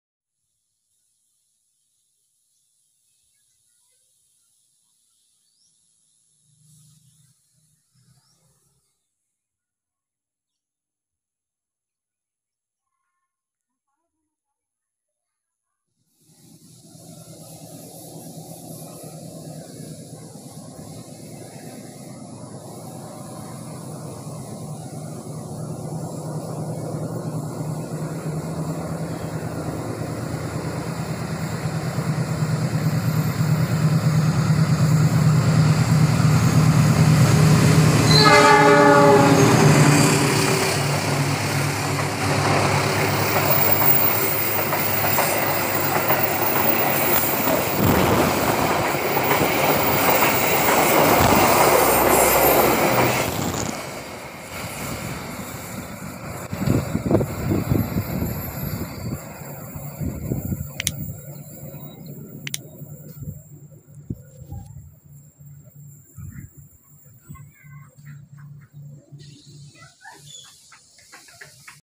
CC 206 13 55 CPN KERETA API ARGO PARAHYANGAN MELIUK DI TIKUNGAN TAJAM CISALADA PURWAKARTA
CC 2061355 CPN ka 38 Argo Parahyangan Di Tikungan Cisalada Purwakarta Relasi Jakarta Gambir - Bandung